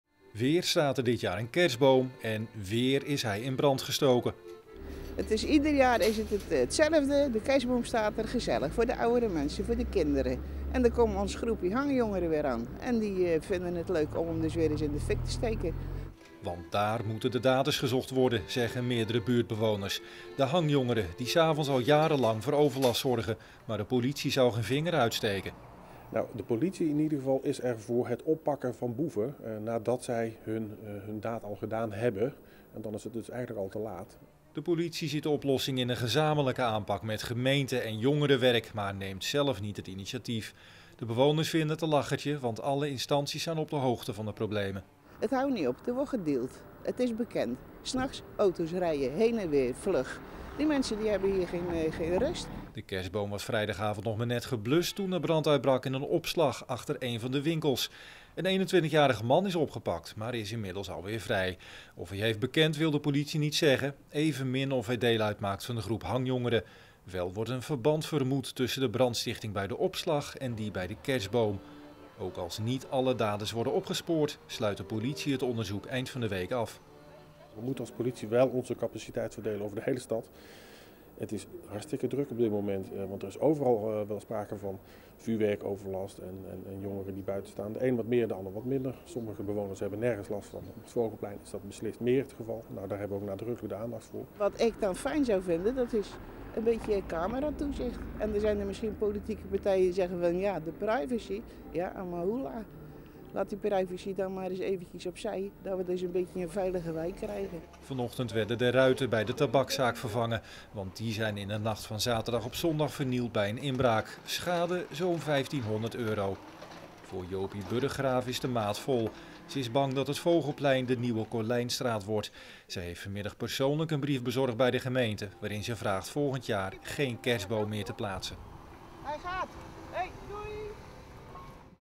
Kerstboom Vogelplein weer in brand gestoken
Het plaatsen van kerstbomen in de wijken is een traditie. Dat binnen een paar dagen de boom op het Vogelplein in brand vliegt ook. Afgelopen vrijdagavond was het weer zover. Bewoners en winkeliers zijn de brandstichtingen en vernielingen helemaal zat.